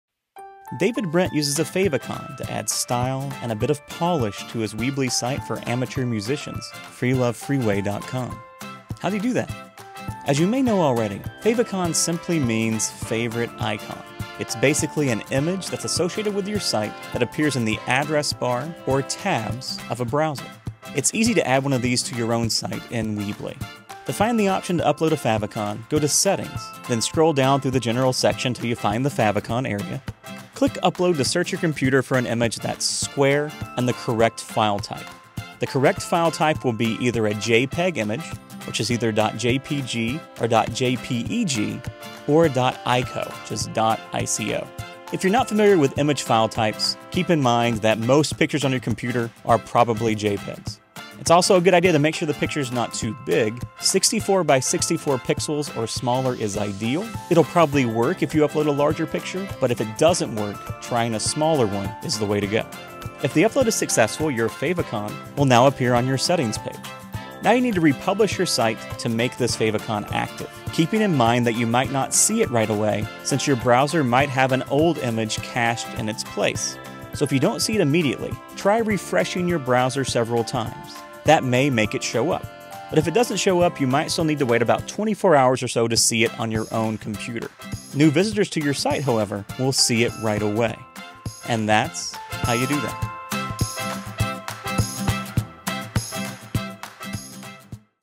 How'd They Do That? Add a Favicon
Learn how to add a Favicon to a Weebly website, blog or online store.